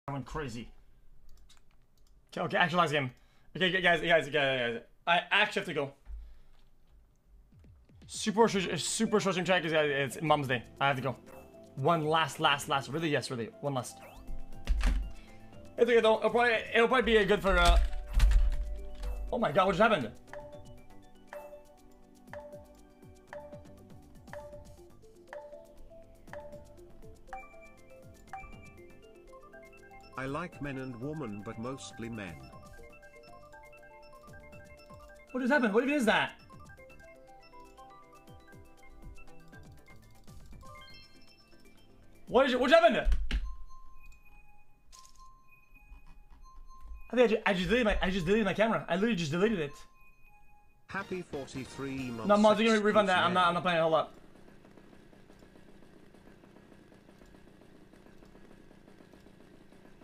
xqc accidentally deletes camera on stream (HACKED)
xqc deletes his camera on mothers day, how crazy